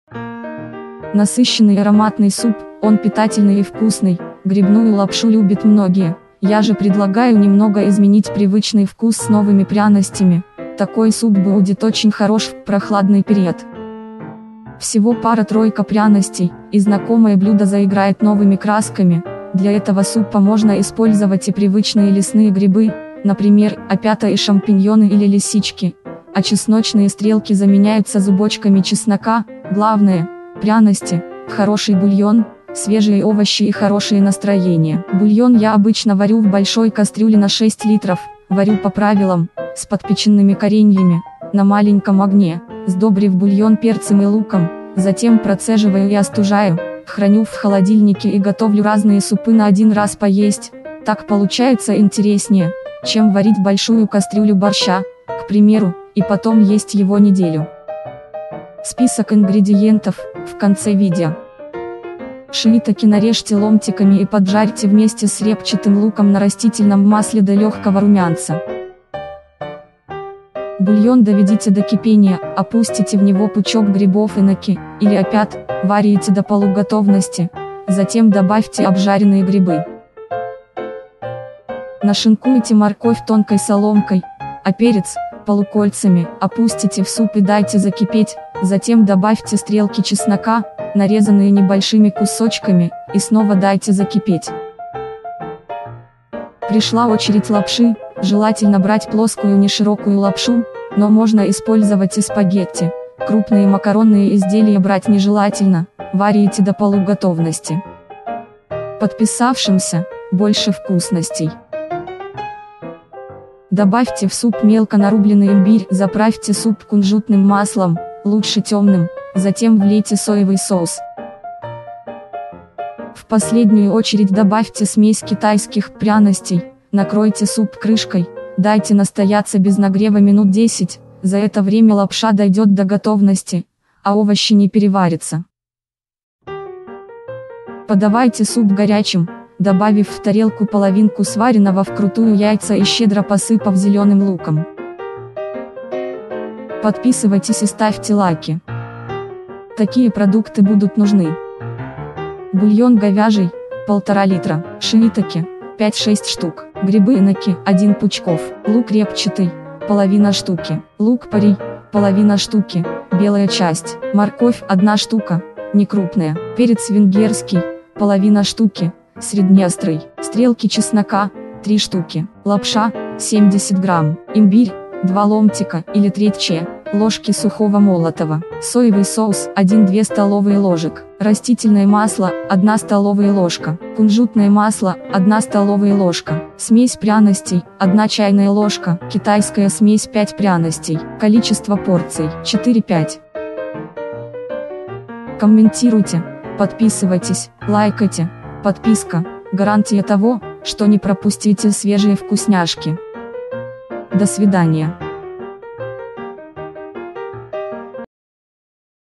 ★ГОТОВЛЮ ЕГО ПОЧТИ КАЖДЫЙ ДЕНЬ!★ ГРИБНАЯ ЛАПША НА КИТАЙСКИЙ МАНЕР ★
★Восхитительный рецепт● Насыщенный ароматный суп, он питательный и вкусный● Грибную лапшу любят многие, я же предлагаю немного изменить привычный вкус с новыми пряностями● Такой суп будет очень хорош в прохладный период●